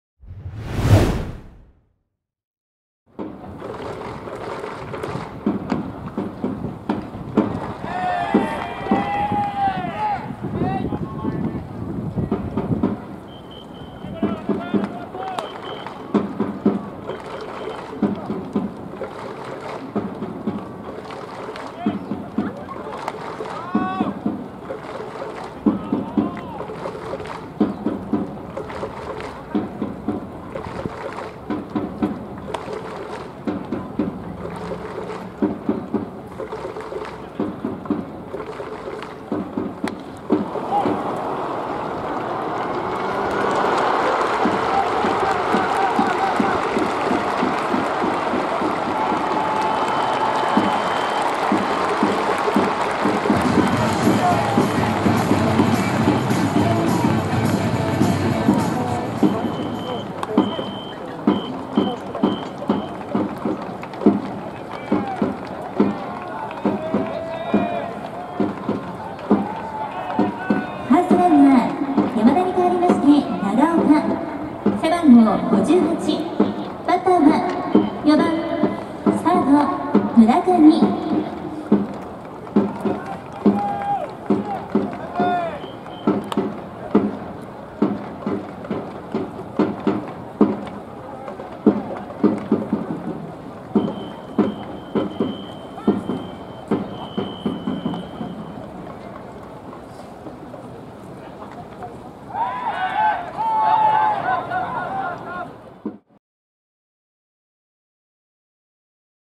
2022年3月11日 #1 山田哲人選手「決勝打となる2点タイムリー」
2022年3月11日

山田哲人
タイムリーヒット